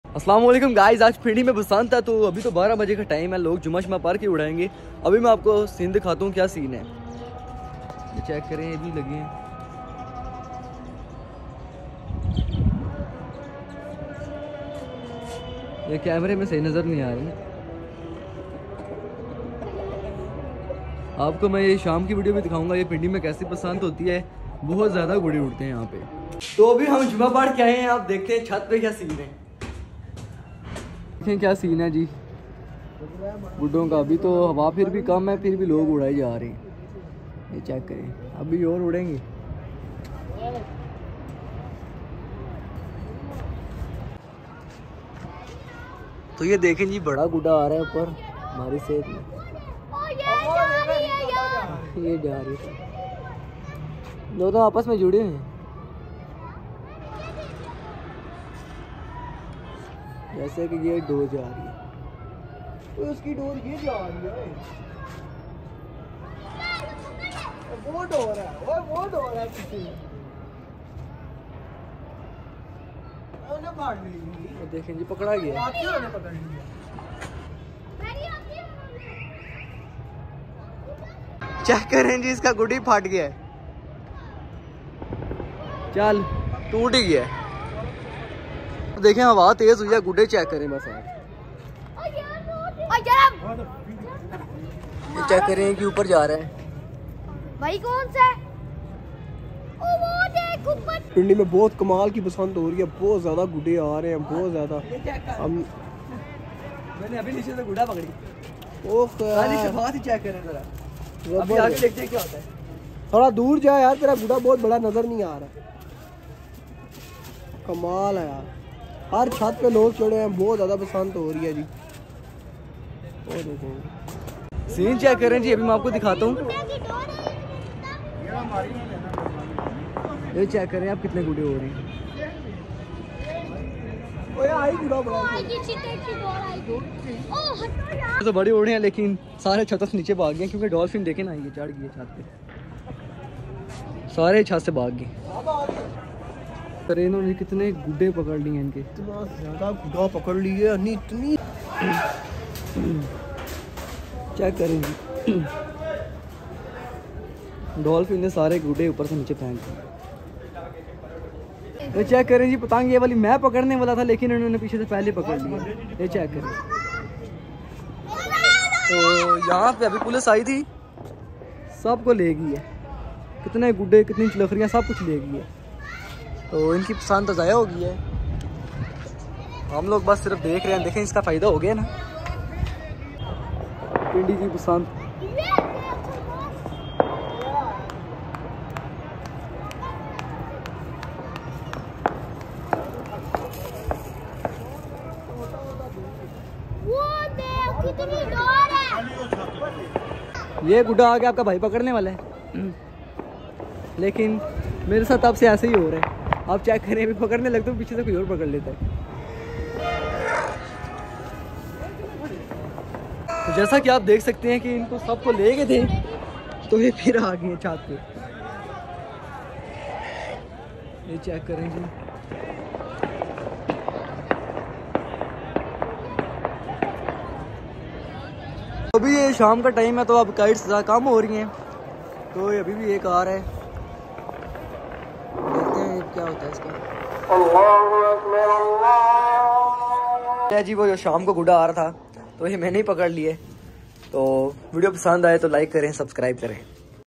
Rawalpindi basant Police agaye police ka chapa 😭😳
Pindibasant2023 starts tonight
Rawalpindibasantnight is on full swing.
Administration try his best to stop but couldn't successful till now.